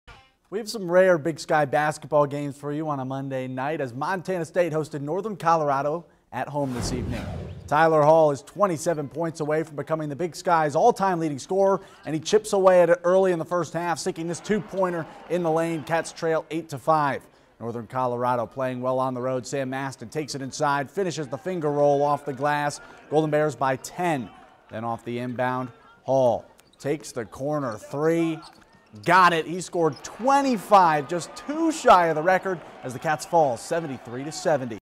MSU narrowly loses to Northern Colorado
In Tyler Hall's near record breaking performance, Montana State falls to Northern Colorado at home.